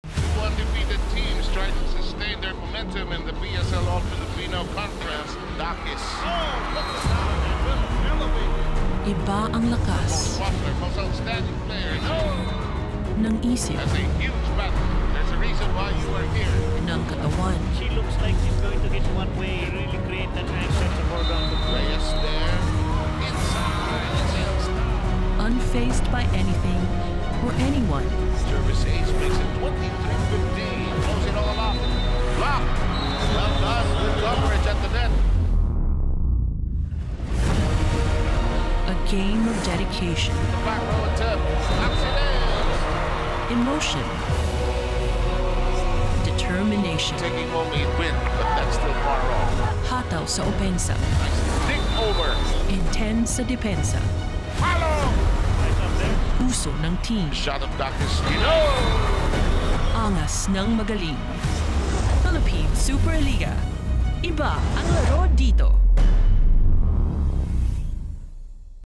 PSL Grand Prix 2017 this October on Aksyon TV International
The PSL Grand Prix Battle begins this October. Who will reign as queens of the court?